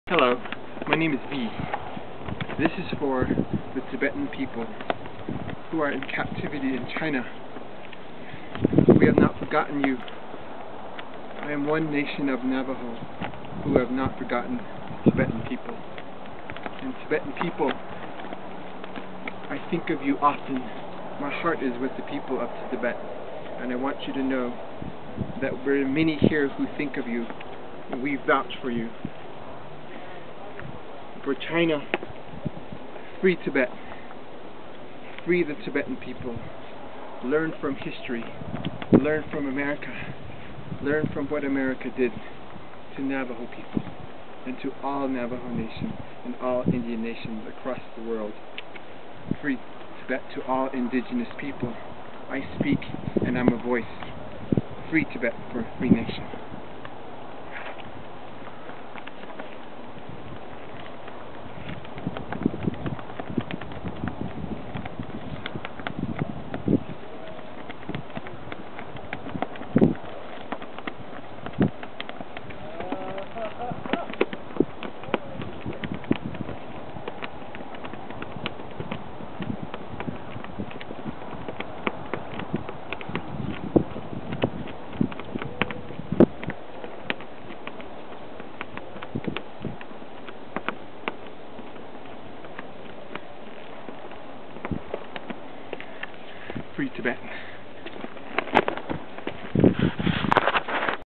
Navajo/Dine' of One for a Free Tibet Nation
My heart is with all Tibetan people held in captivity by China.
China...Learn from America and what it did to Native American People.  China FREE TIBET!!